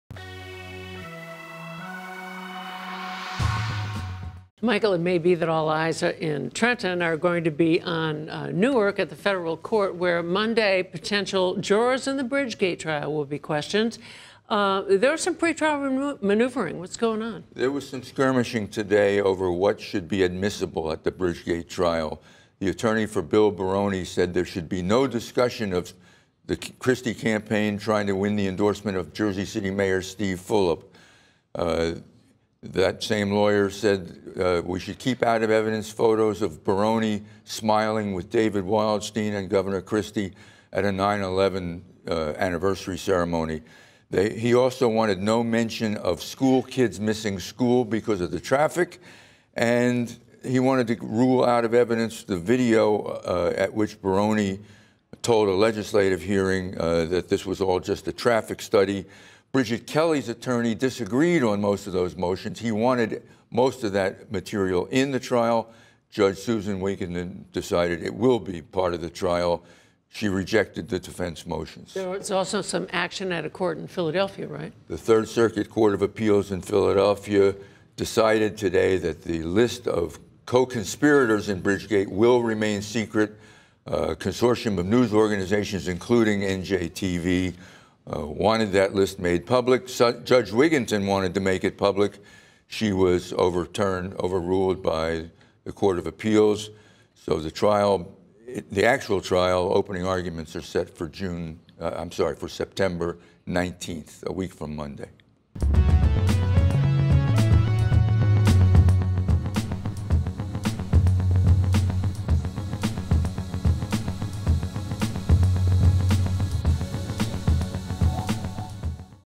Bridgegate Co-Conspirators to Remain Secret
The Third Circuit Court of Appeals in Philadelphia decided that the list of co-conspirators in Bridgegate will remain secret. Potential jurors will be questioned Sept. 12 and the trial is scheduled to begin Sept. 19.